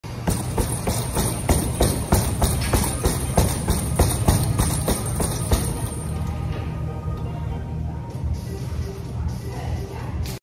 Tabata Tuesday at The Swole
Tabata Tuesday with boxing, burpees, farmers, wall ball SitUps